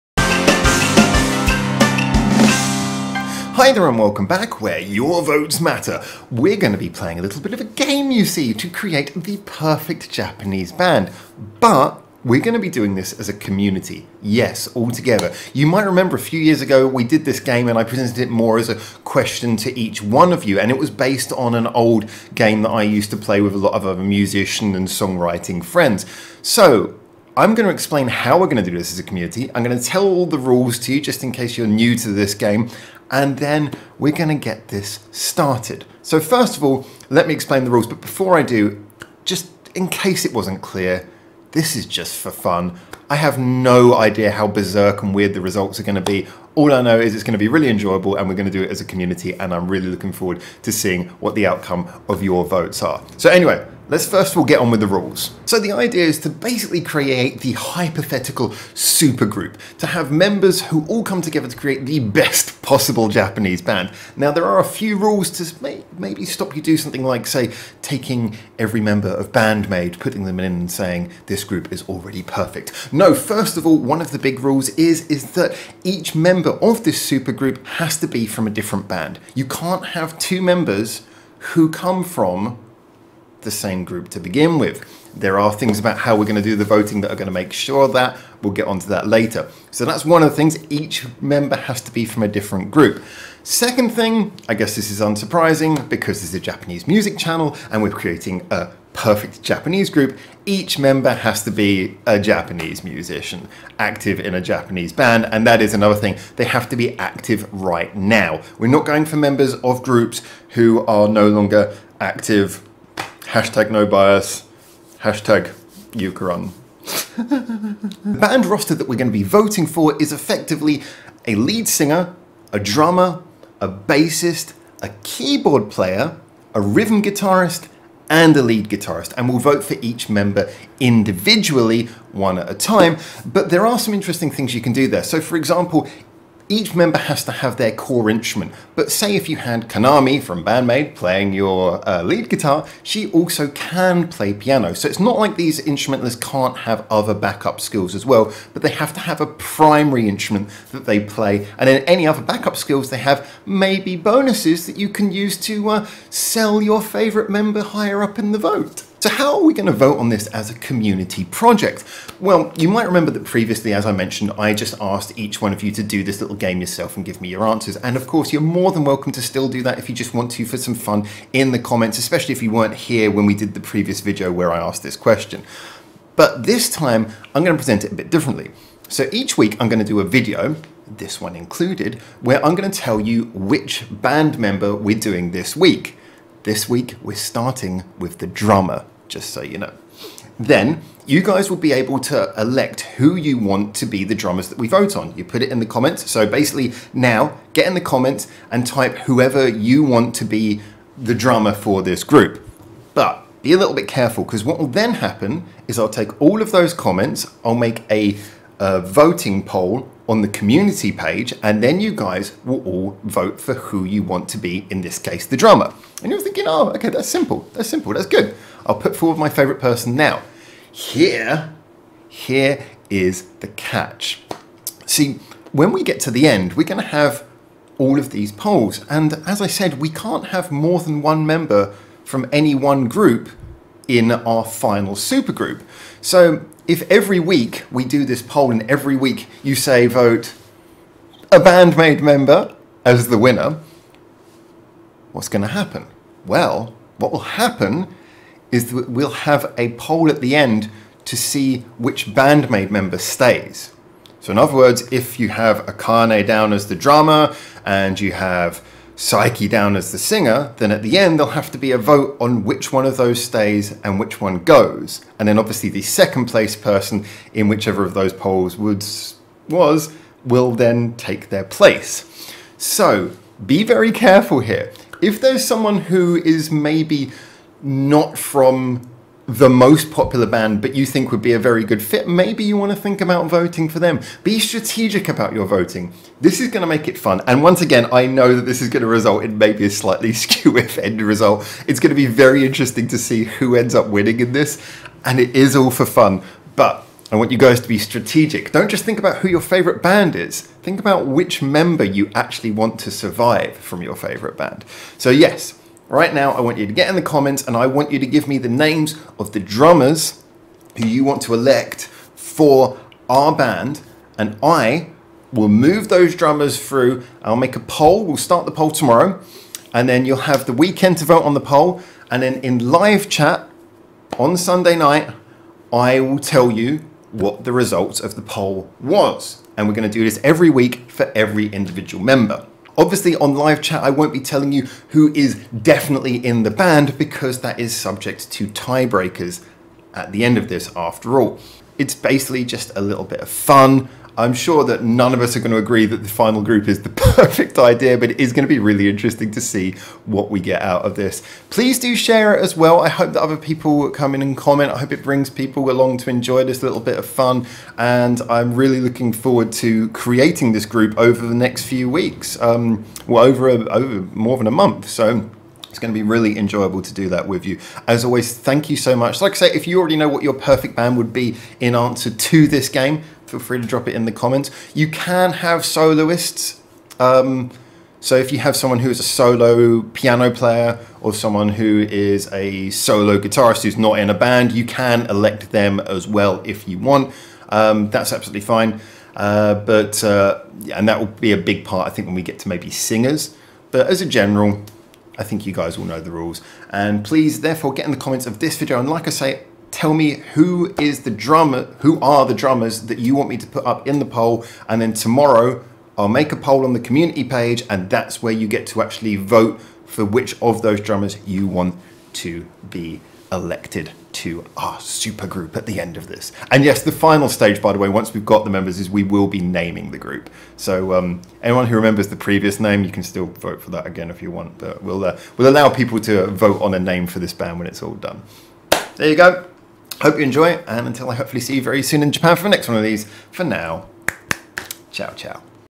LET'S MAKE A "PERFECT" BAND! (... and you decide the members)
*** VOTING NOW CLOSED ***
BAND-MAID 's Misa on bass?? SCANDAL 's Mami on guitar?? LOVEBITE 's Haruna on drums?? ...Time for us to decide!
This project will take a number of weeks, but should be really fun! This video explains how the voting will work, what the rules are, and starts the first round.
Please enjoy, it is all for fun!
@0:00 Intro
@0:55 The objective & rules
@2:38 How we will vote
@5:04 Final thoughts & notes
If you like this content, please consider the following :)